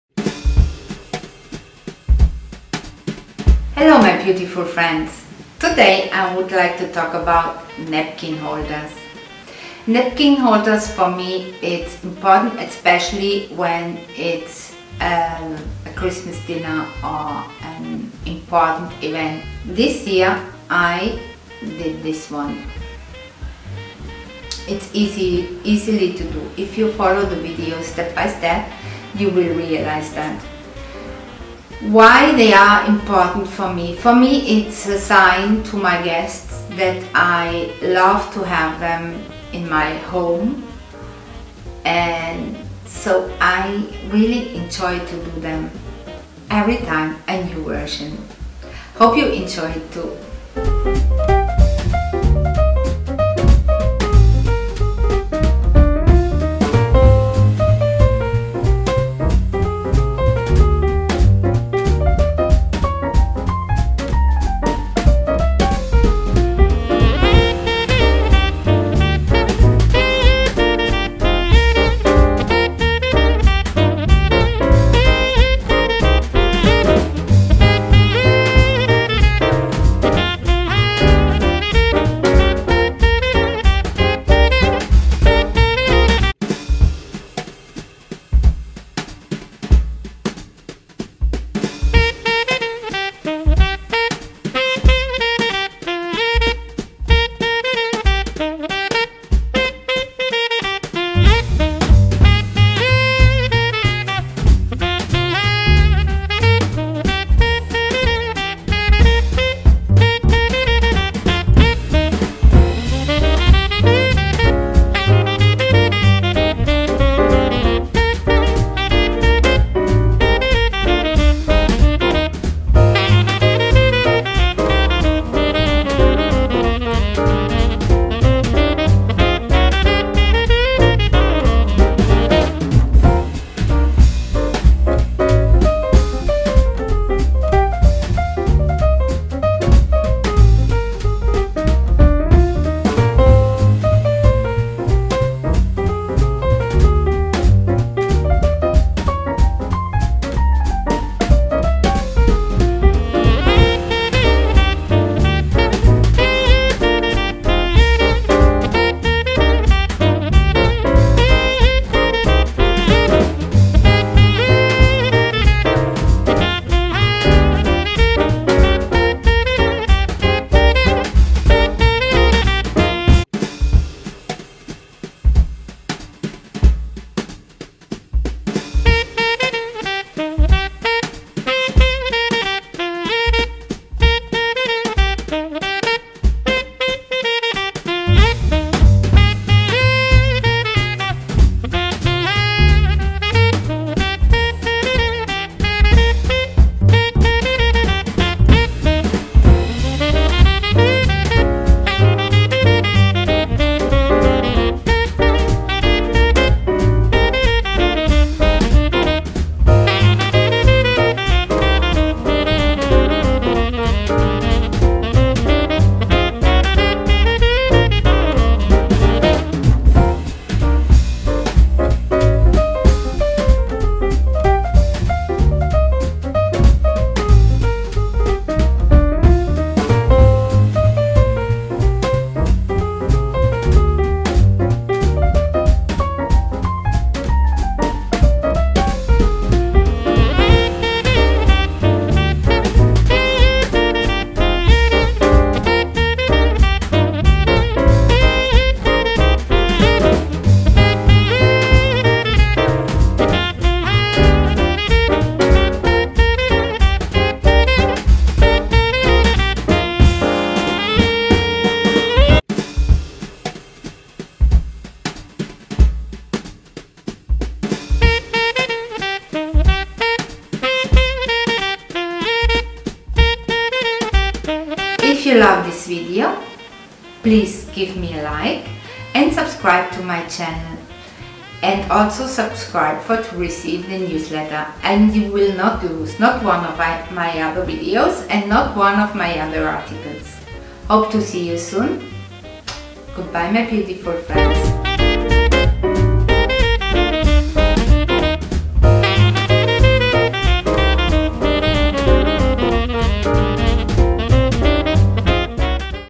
DIY Napkin Holder
Hello my beautiful friends! Christmas is a period in the year that we love celebrating and we usually organize our Christmas dinner long time in advance to get everything perfect. “Perfect” for me means to have napkin holders,...contnue on my website!